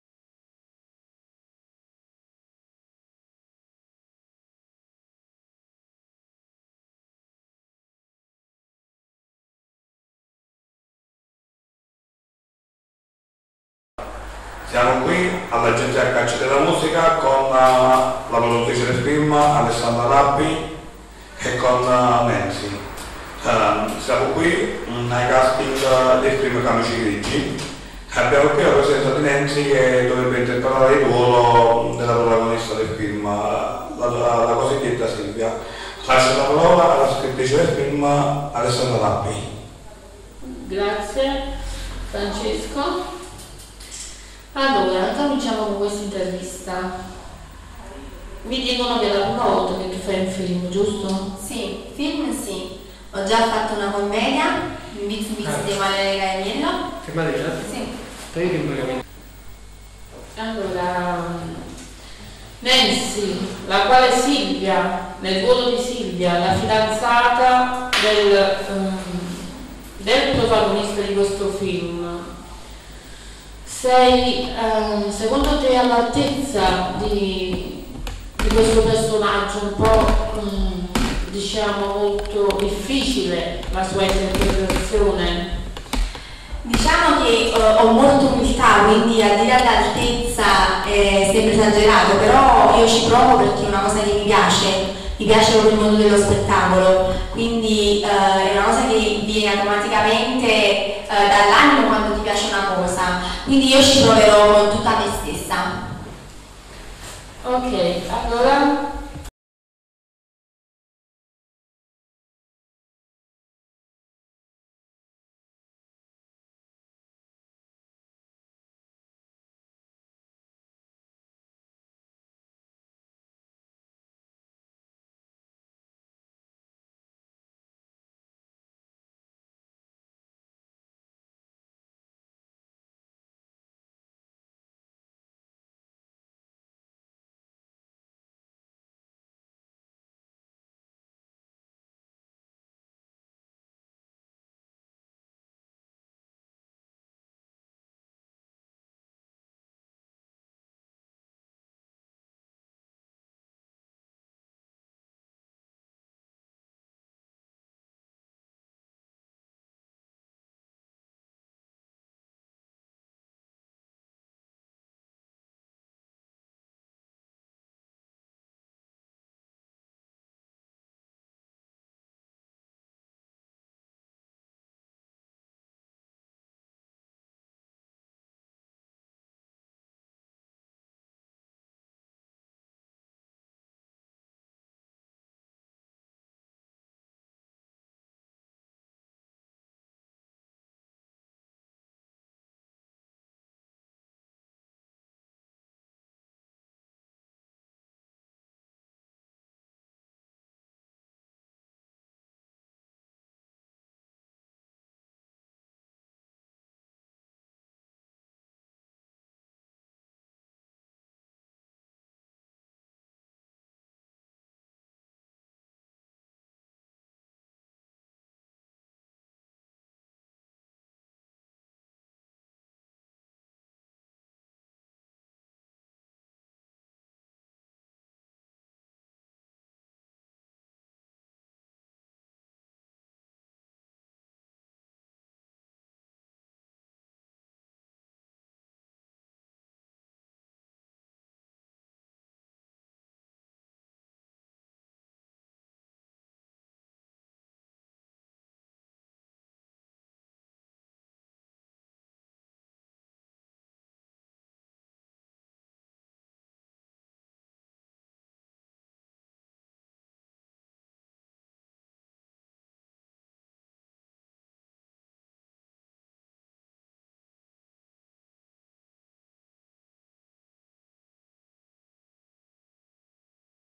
Nancy Grazioso Intervista camici Grigi .
Francwesco Bortone ed Alessandra Nappi intervistano Nancy Grazioso, per il Film Camici Grigi .